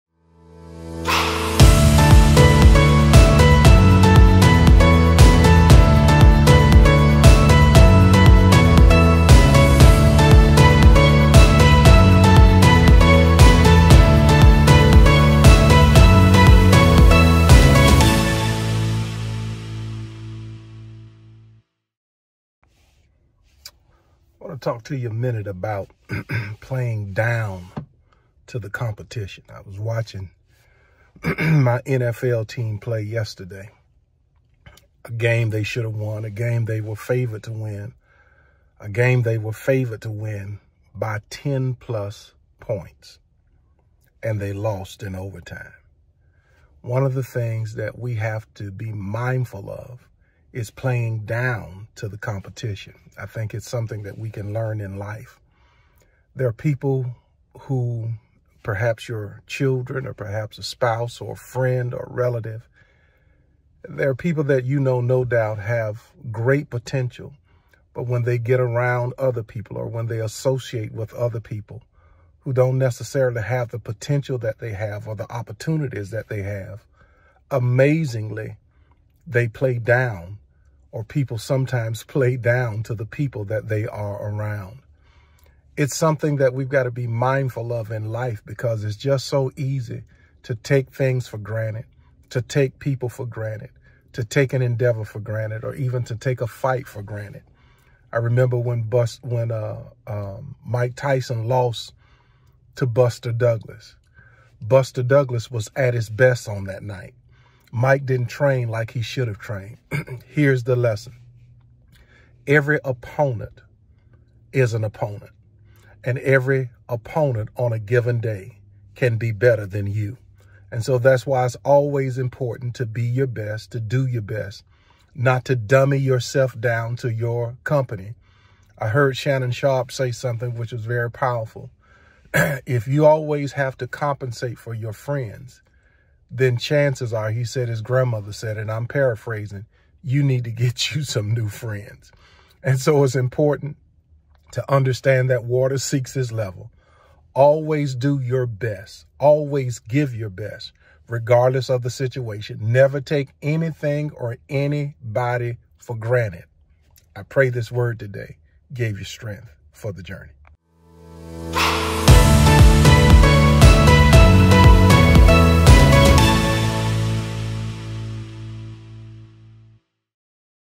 Playing Down To The Competition | Strength For The Journey | Pastor Gary Williams
Are you playing down to the competition? Do you know someone who has great potential but not living up to it? This is a powerful and must-watch devotional message you don't want to miss. Join Pastor Gary Williams on Thursday | 7PM for Strength For The Journey.